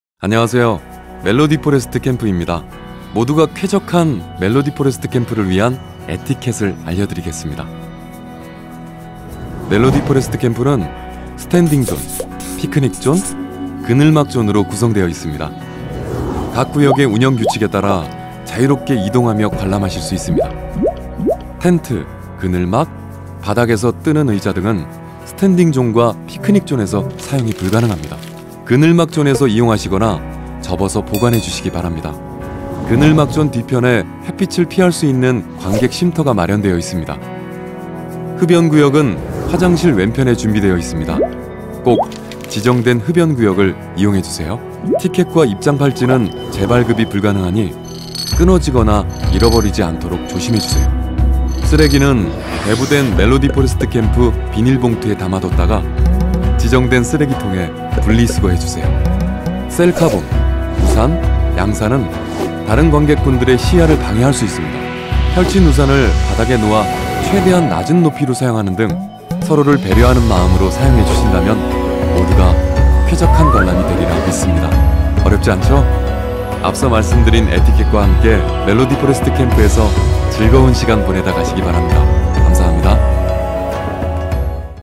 멜로디 포레스트 캠프 에티켓 안내!
안녕하세요. 멜로디 포레스트 캠프입니다.
모두가 쾌적한 멜로디 포레스트 캠프를 위한 에티켓을 알려드리겠습니다 :)